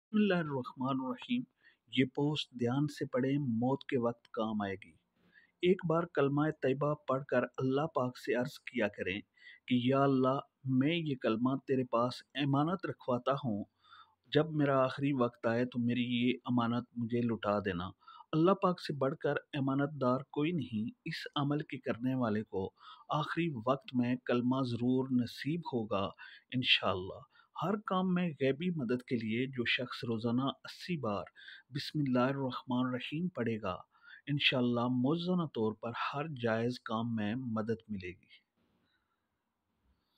MOOT KY WAQAT KAM ANY WALA AMAL /موت کے وقت کام آنے والا عمل / dailyDua/Abid raja Islamic Channel
بسم اللہ الرحمن الرحیم جب بھی کسی انسان کے موت کا وقت آتا ہے اللہ تعالی کا کام ہے کہ اس انسان کے مقدر میں کیا ہوگا اسے مرتے وقت کلمہ نصیب ہوگا یا نہیں اس لیے کوشش یہ کرنی چاہیے کہ موت سے پہلے اپنے اعمال کو درست کرلینا چاہیے اور اللہ تعالی کے احکامات کو جتنا چاہیے اور ان پر عمل درآمد بھی کرنا چاہیے کیونکہ موت کا وقت بہت مشکل ہوتا ہے اس وقت اگر آپ کلمہ نہیں پڑھا ئیں گے تو آپ جن میں ہی ہوں